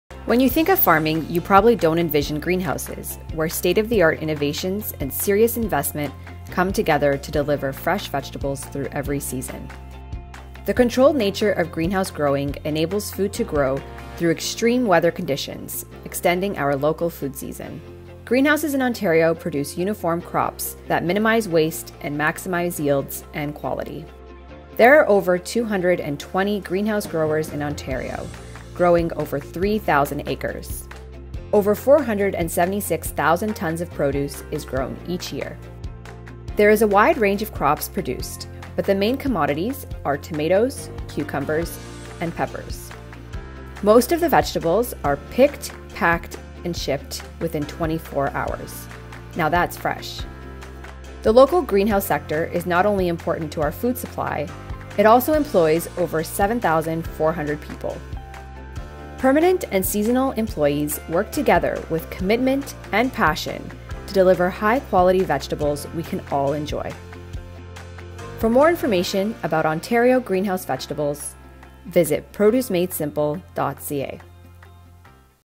All About Ontario Greenhouses| Produce Made Simple
Love local! When you think of farming, do you think of the innovation and state of the art facilities of greenhouses? Watch to learn about Ontario greenhouses